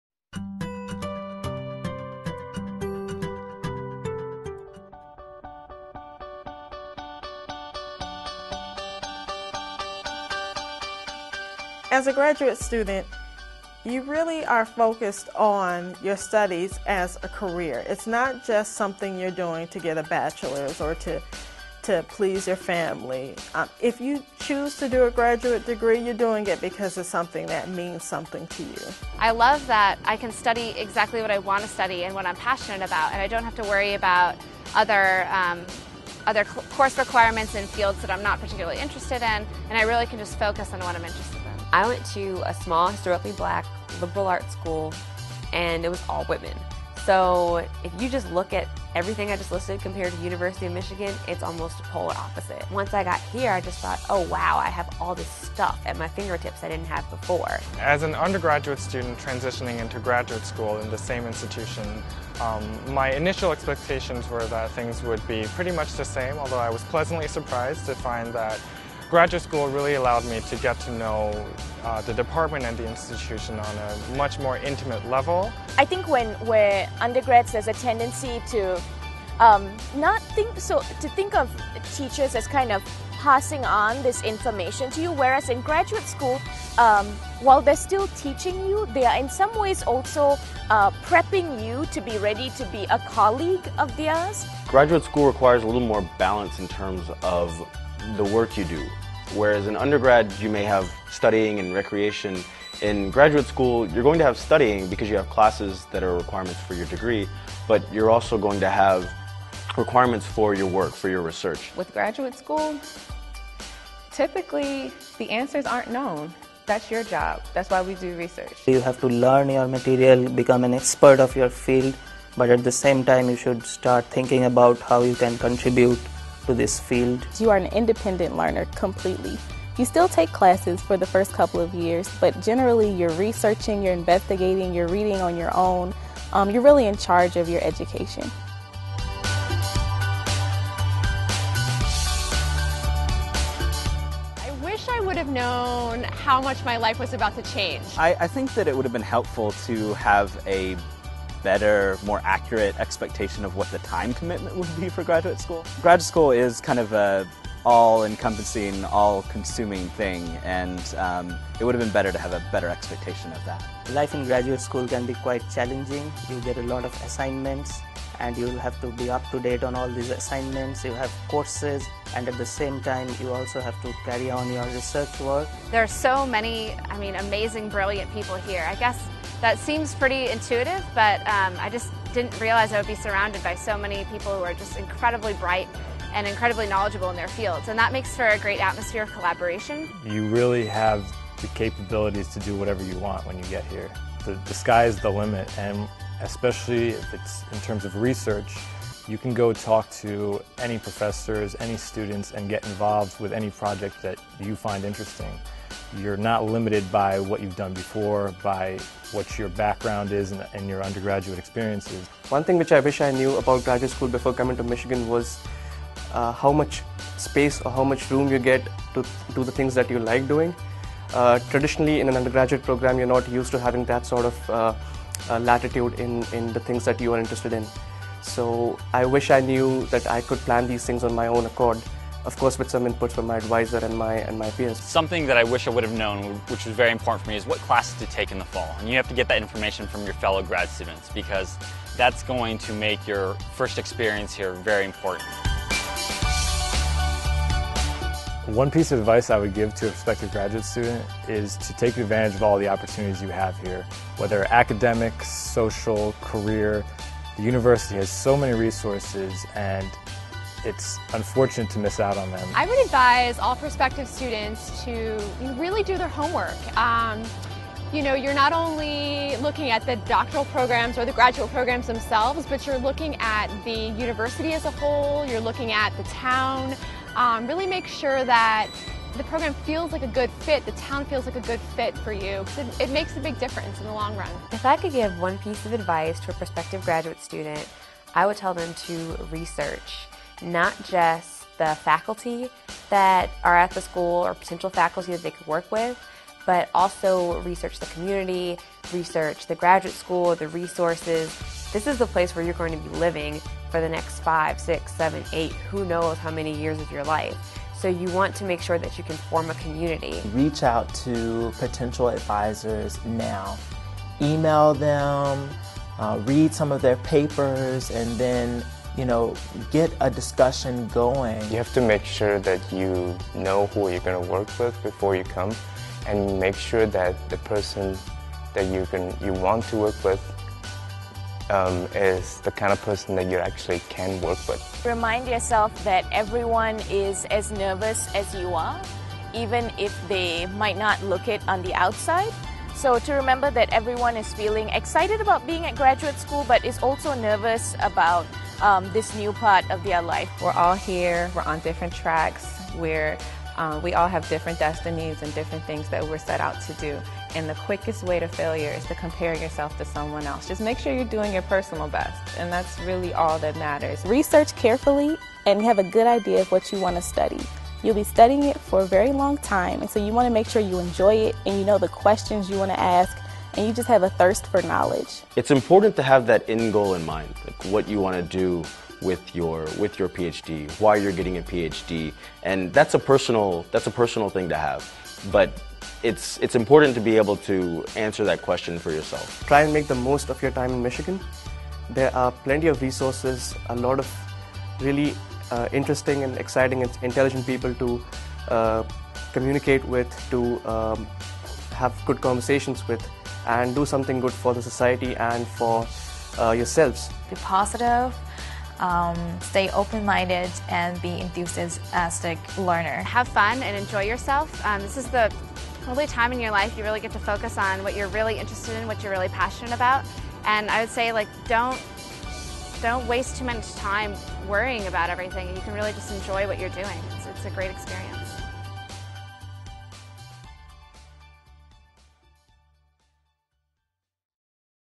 Getting Ready for Graduate School
As you prepare for graduate school, there are some important things to keep in mind.  Graduate students explain how they think graduate school is different than their undergraduate experience, what they wish they would have known before coming to graduate school, and offer advice for new and prospective graduate students.